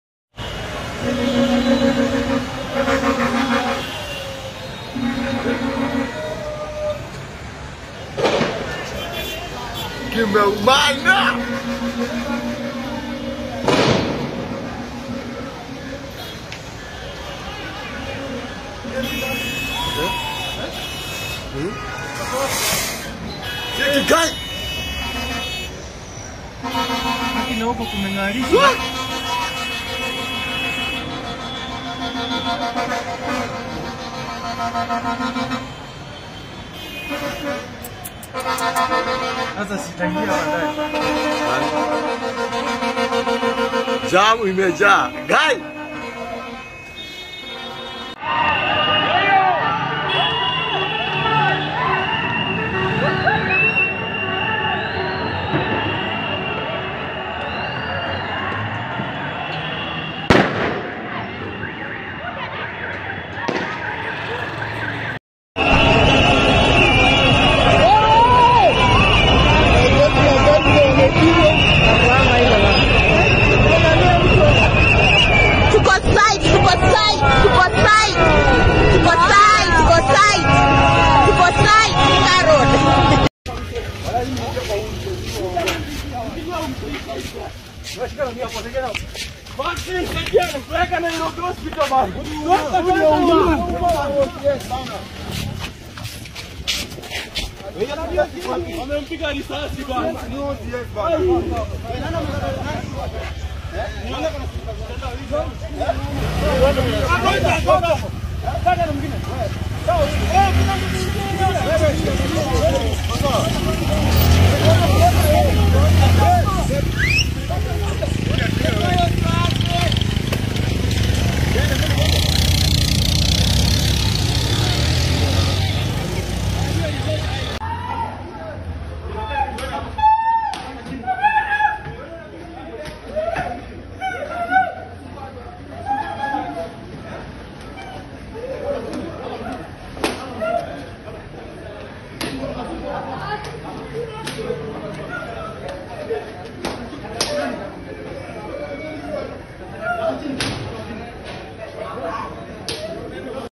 Kimeumana, azimio demonstrators engage police officers in Nairobi
Azimio la Umoja mass action demonstrations